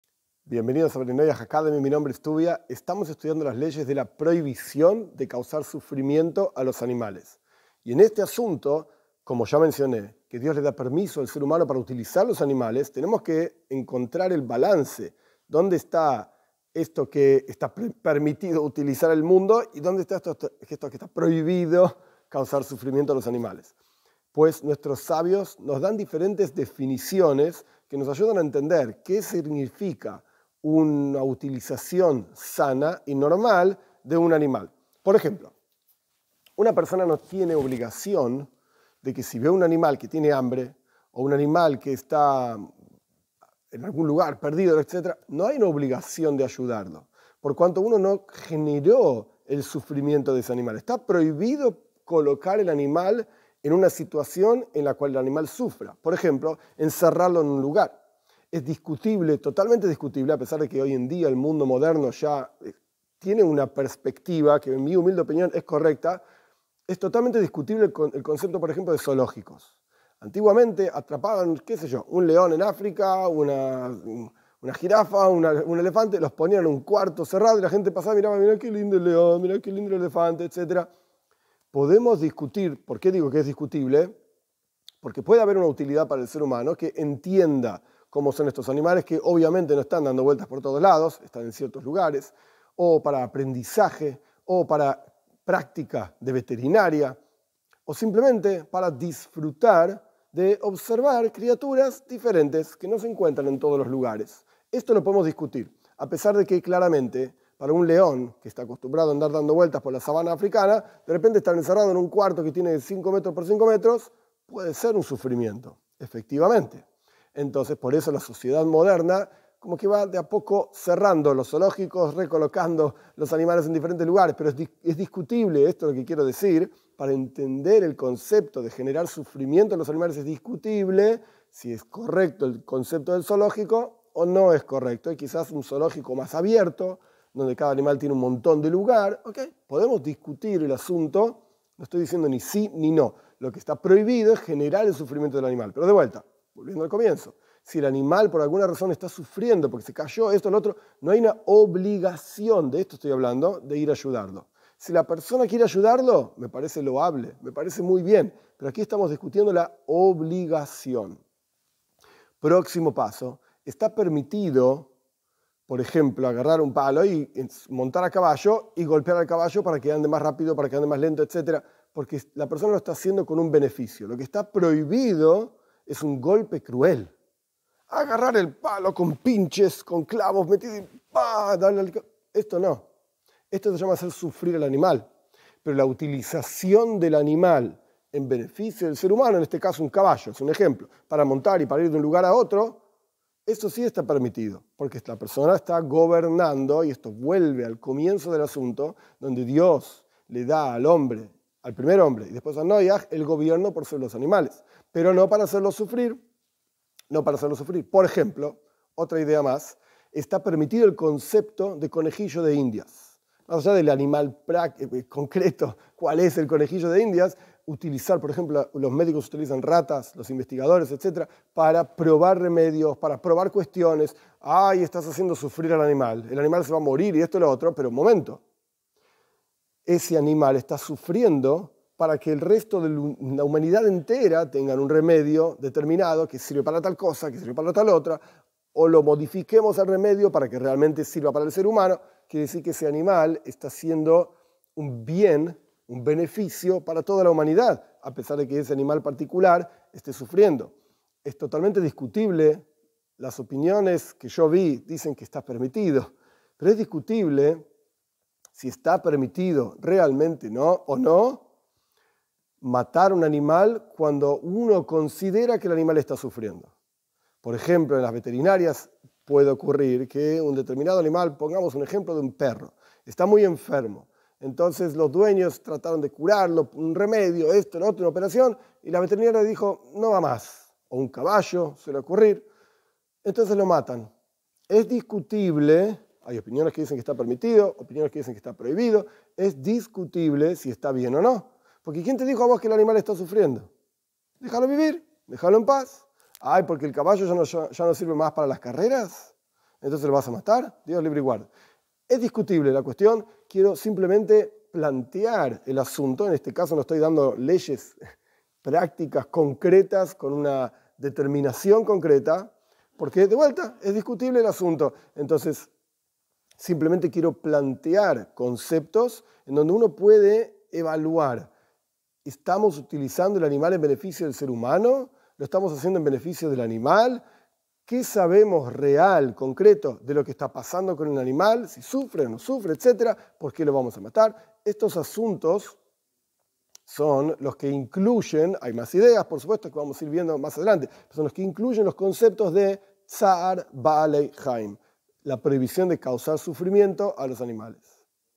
Chispas de Torá #86 - Rabino Tuvia Serber
¿Qué sabes sobre Bnei Noaj? Pues es hora de aprender...